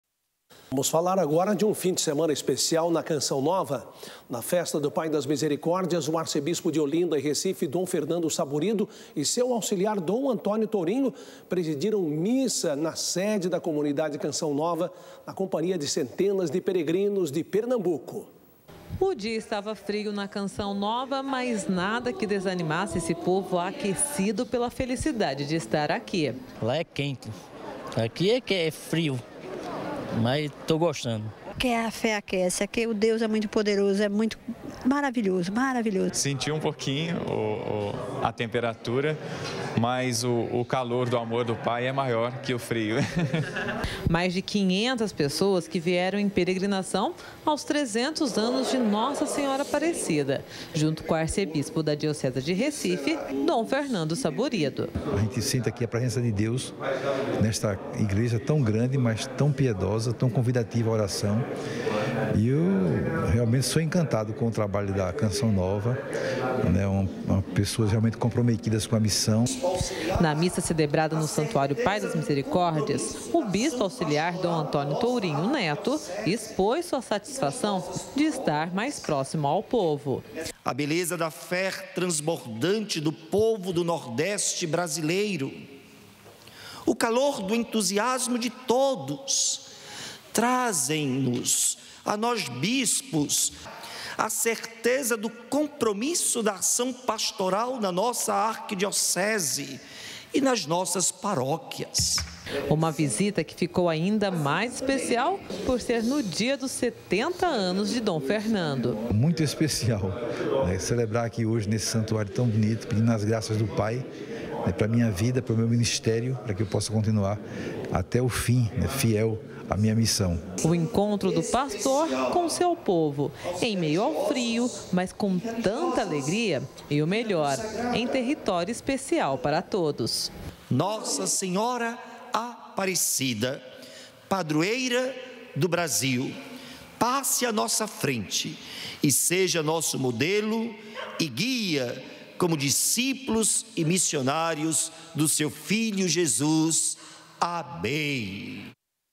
Bispos e fiéis de Recife visitam a Canção Nova
O final de semana na Canção Nova que foi especial. Na Festa do Pai das Misericórdias, o arcebispo de Olinda e Recife, Dom Fernando Saburido, e seu auxiliar Dom Antonio Tourinho, participaram presidiram missa por aqui na companhia de centenas de peregrinos de Pernambuco